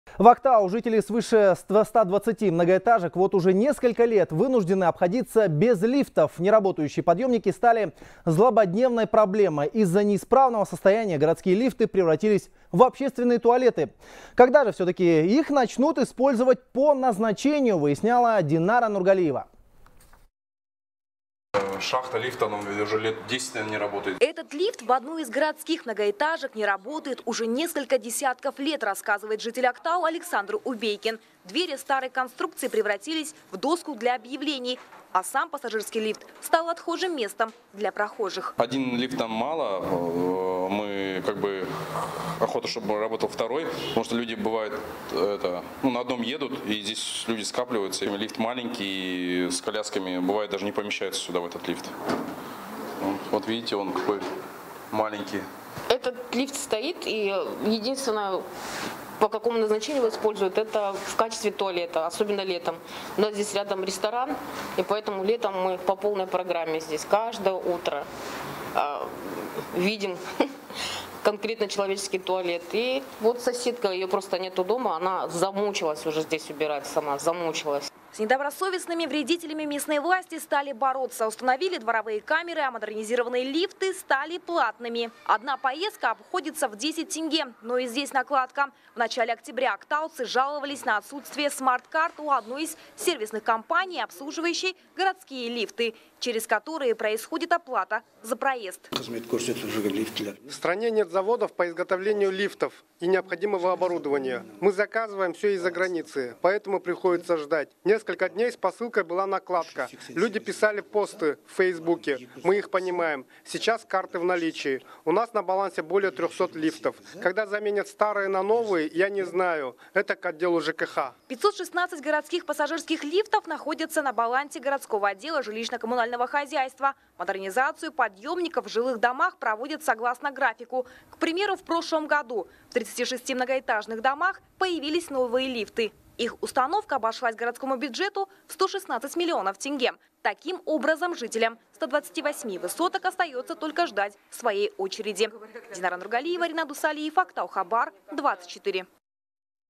Более 120 лифтов нуждаются в замене в Актау
В Актау жители свыше 120-ти многоэтажек уже несколько лет вынуждены обходиться без лифтов. Неработающие подъёмники стали злободневной проблемой. Из-за неисправного состояния городские лифты превратились в общественные туалеты, сообщает корреспондент «Хабар 24».